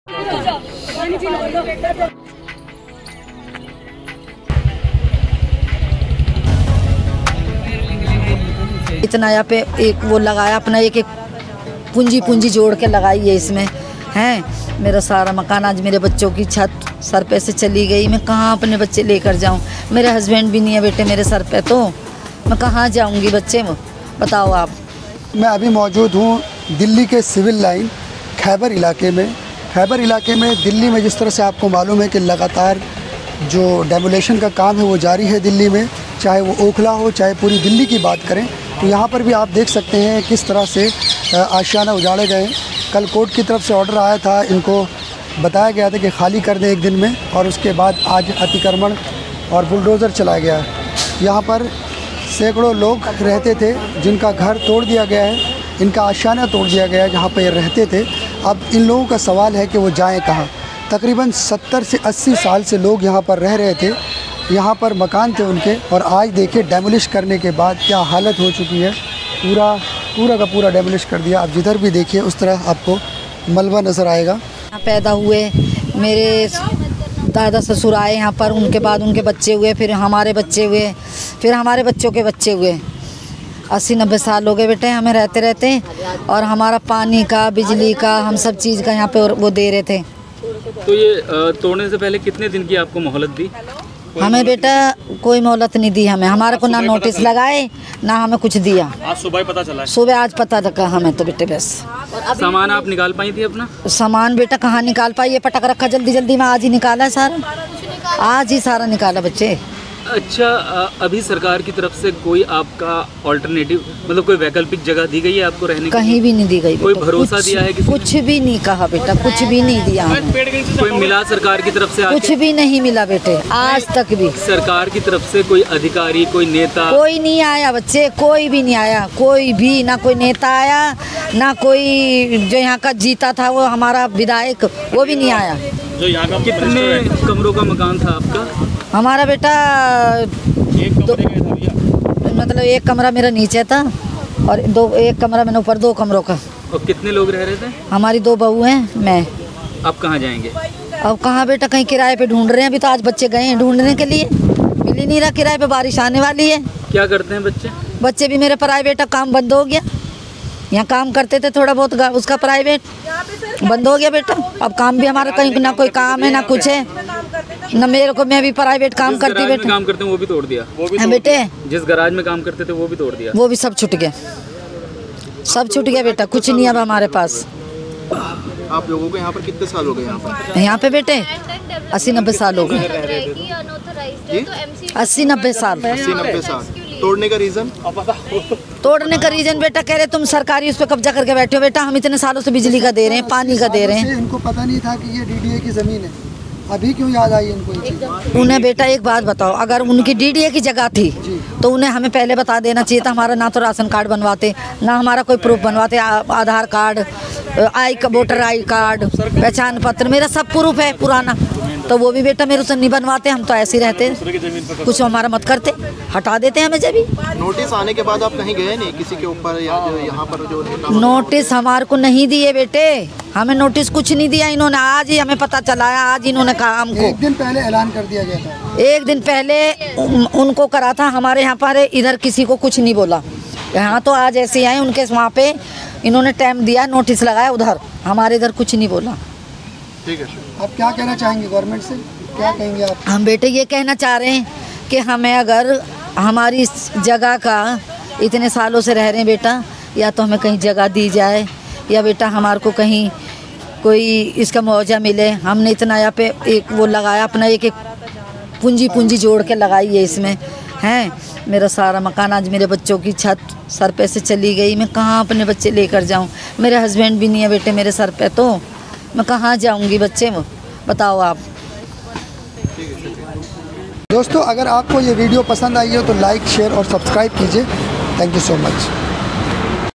Khyber pass demolition | पीढ़ियों से रह रहे लोगों का मकान हुआ जमीं दोज़| लोग रोते रहे घर टूटता रहा
Dekhi Khyber pass demolition |  पीढ़ियों से रह रहे लोगों का मकान हुआ जमीं रोज़ |